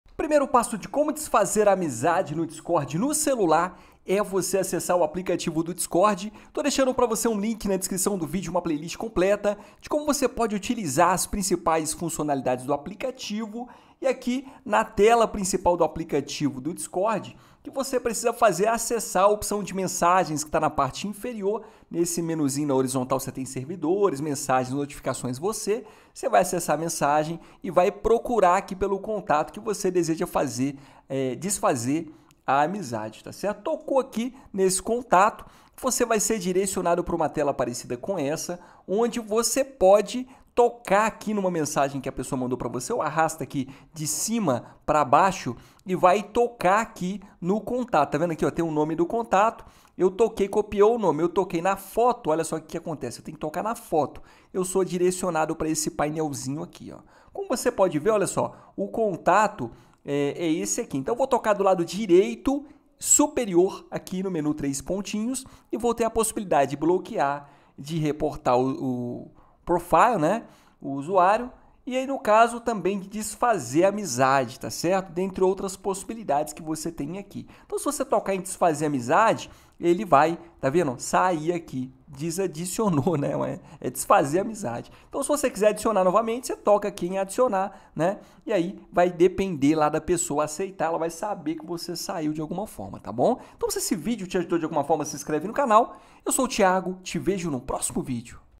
COMO DESFAZER AMIZADE NO DISCORD NO CELULAR
⏩ Neste vídeo, você vai aprender como desfazer amizade no Discord no celular de forma rápida e fácil. O Discord é um aplicativo de comunicação que permite criar e participar de servidores com diferentes temas e interesses. Mas às vezes, você pode querer se afastar de alguém que não te agrada ou que te incomoda. Para isso, basta seguir os passos que vou te mostrar a seguir..